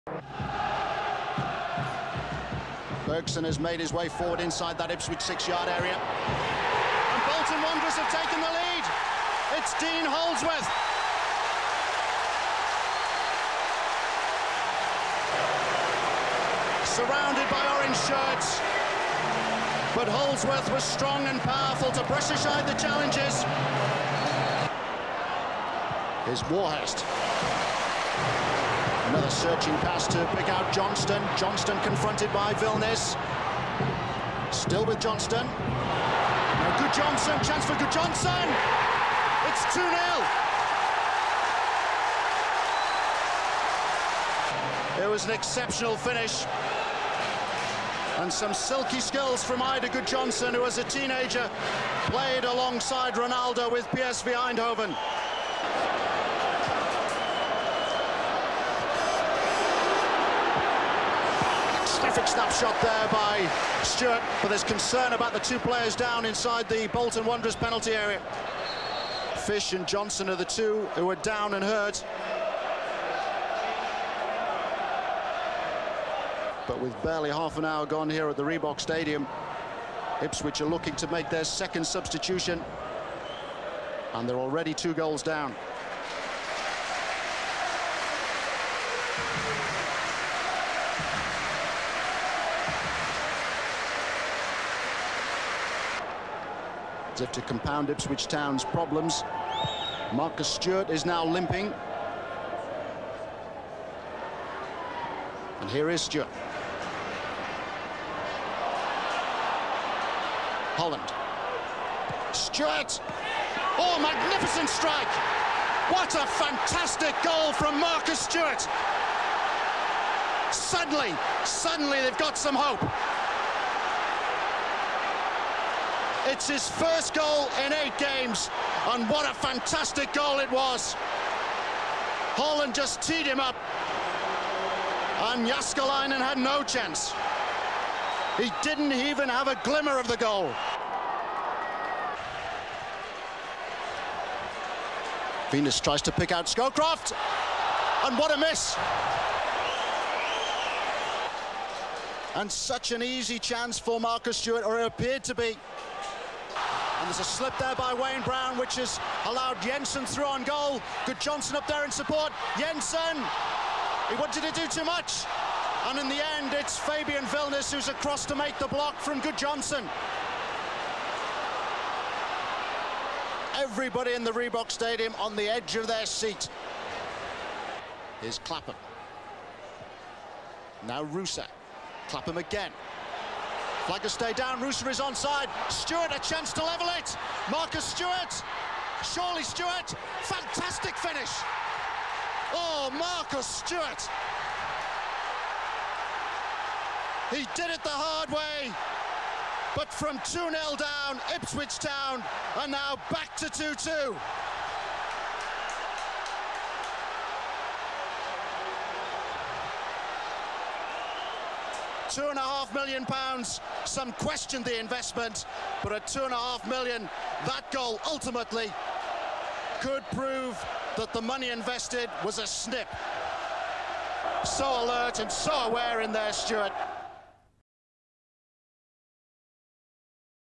20 YEARS ON | BOLTON 2 TOWN 2
Highlights of Town's come-from-behind 2-2 draw at Bolton's Reebok Stadium in the First Division play-off semi-final first leg, 20 years ago today.